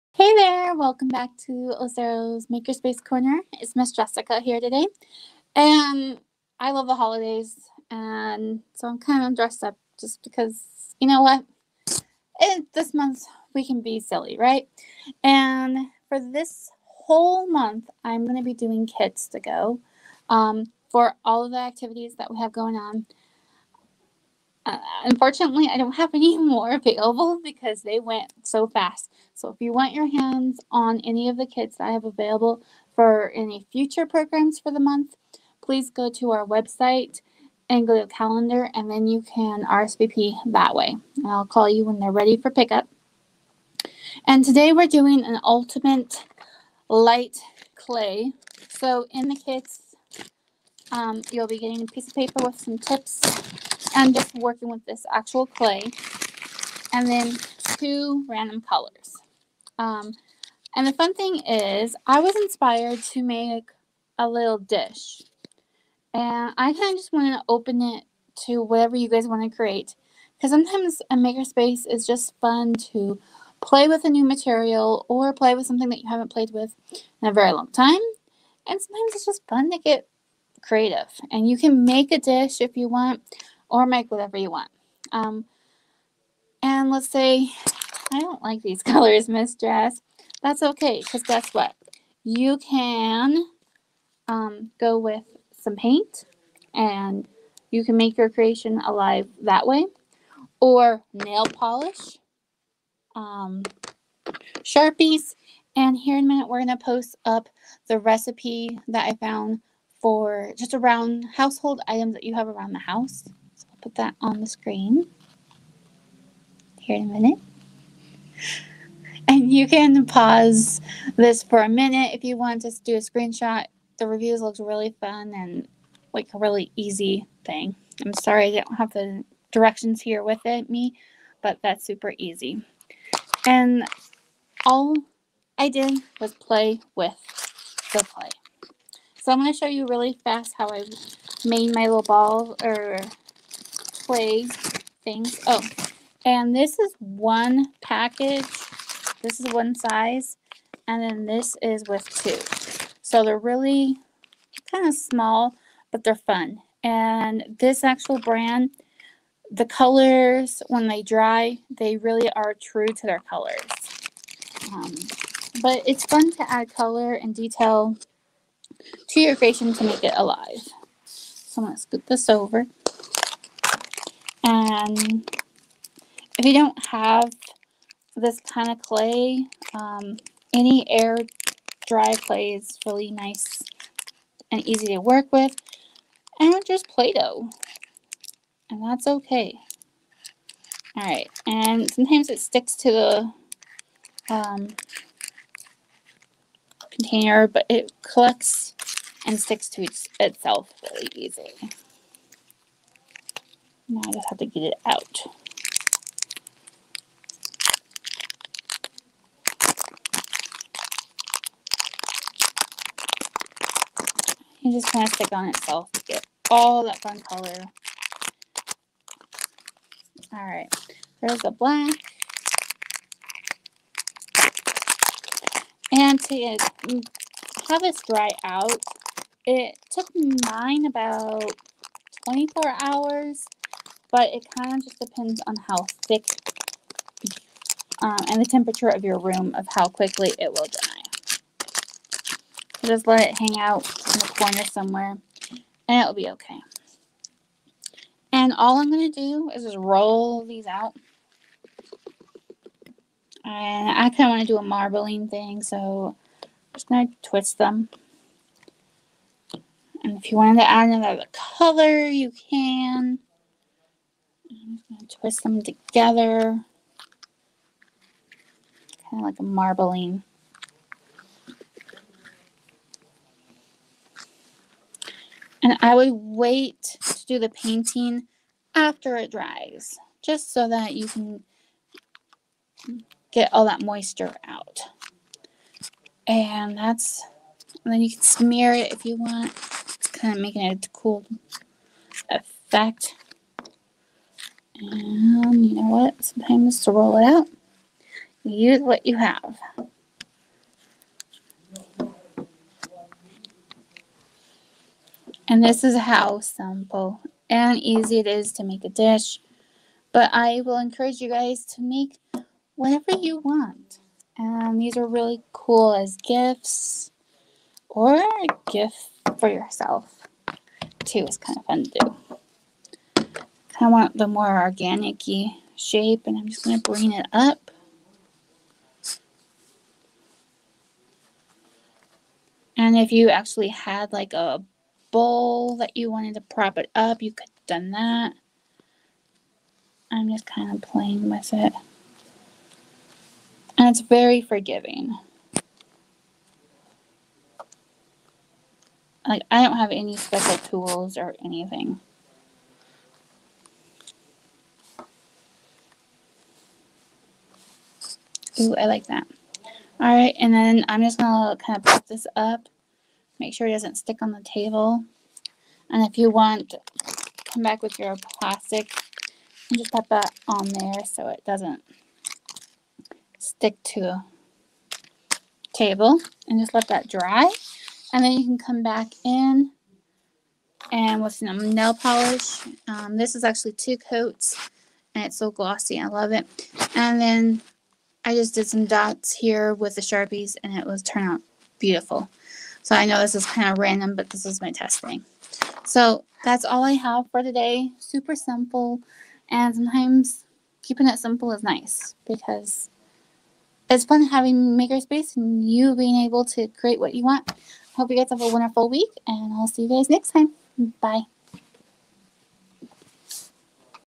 Makerspace Corner: Clay Bowls | Lucero Library
Join Ms. Jessica’s to learn how to make a simple bowl out of air dry clay. Kits are available while supplies last.
Livestreaming on the Lucero Library Facebook and YouTube pages.